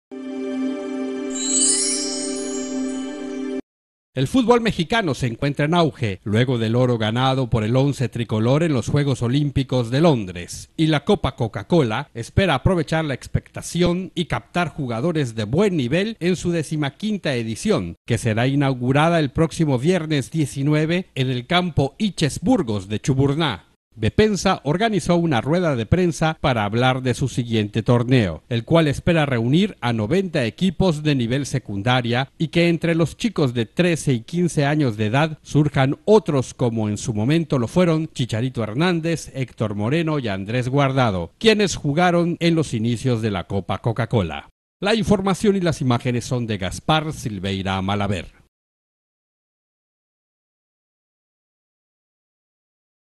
Gran expectación en la Copa Coca Cola
El fútbol mexicano se encuentra en auge luego del oro ganado por el once tricolor en los Juegos Olímpicos de Londres y la Copa Coca Cola espera aprovechar la expectación y captar jugadores de buen nivel en su 15a. edición